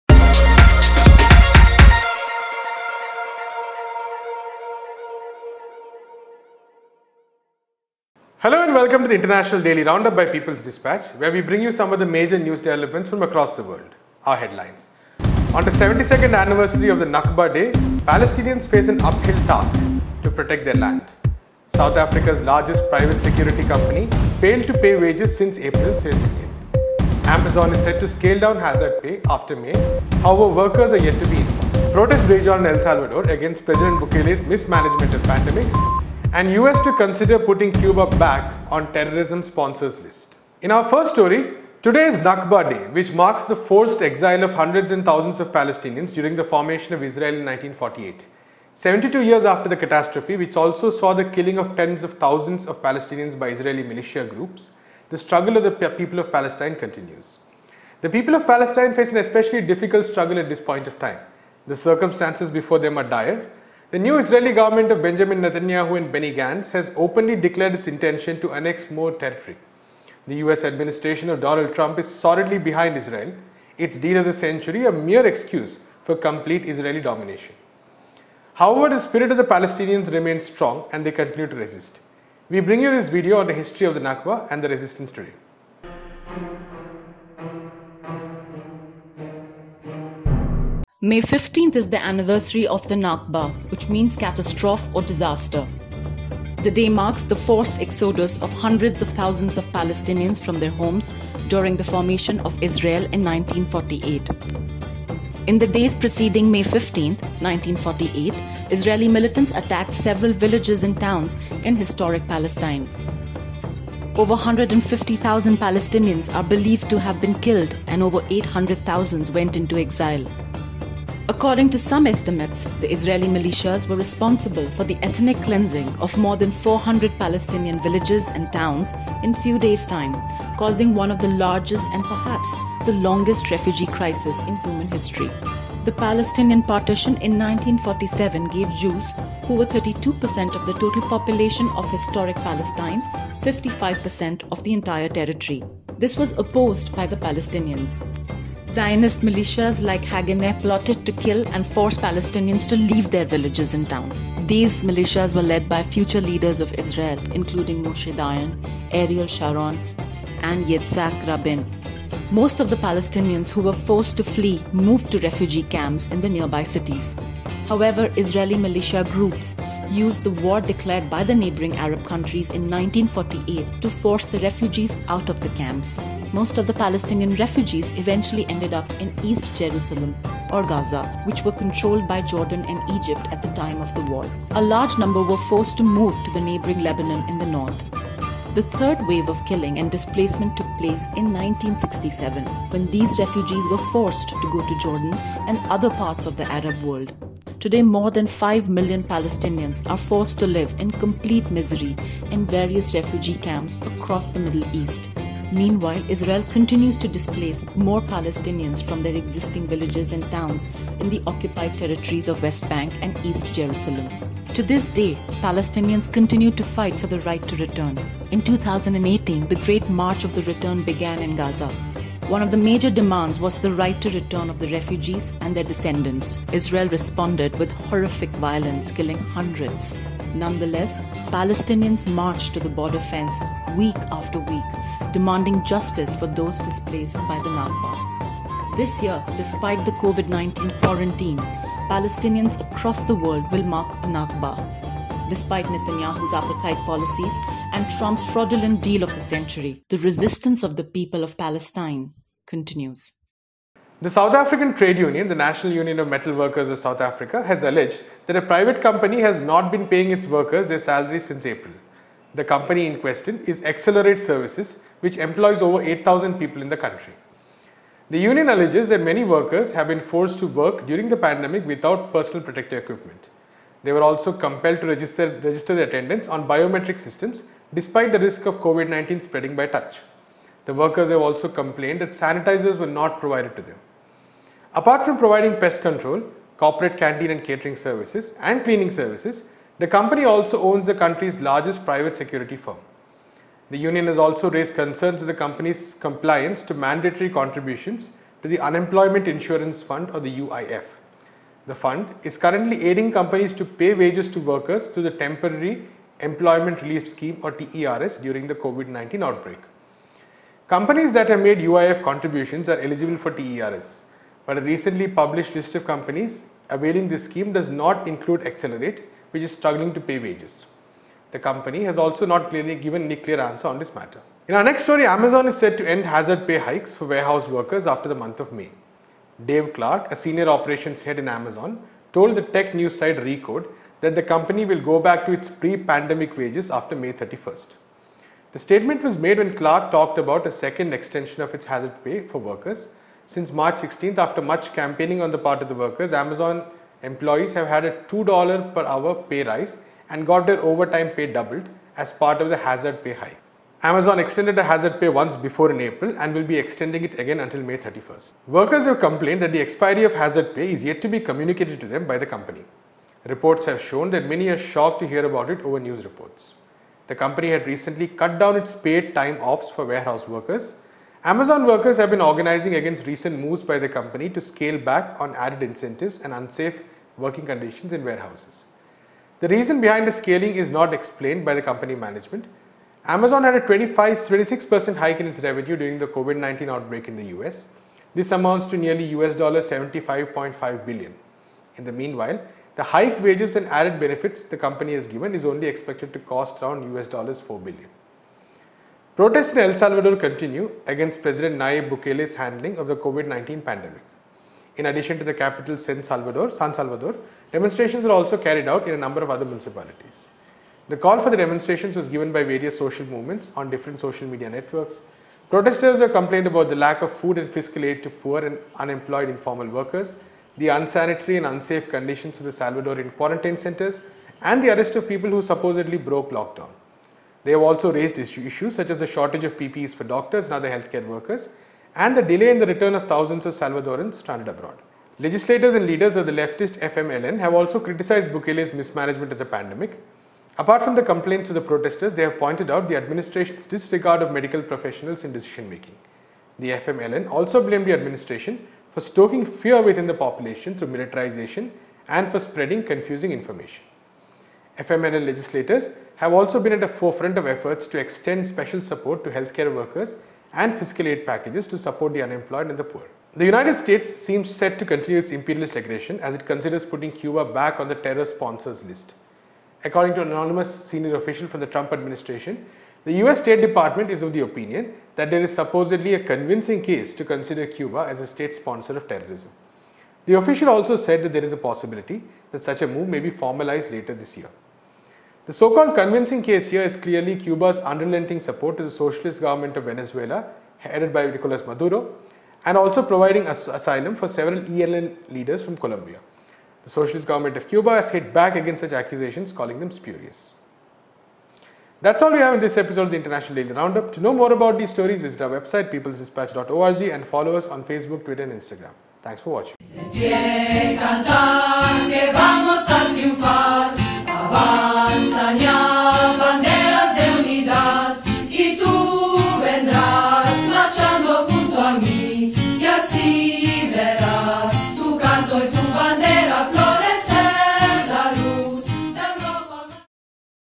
Palestinian Face Uphill Battle on 72nd Nakba Day
On today’s episode we take a look at the looming colonization attempt by a unity government in Israel and the historic importance of the Nakba Day. We also take a look at the news of over 8,000 employees of a private security company in South Africa, not being paid their wages since April and other top news of the day.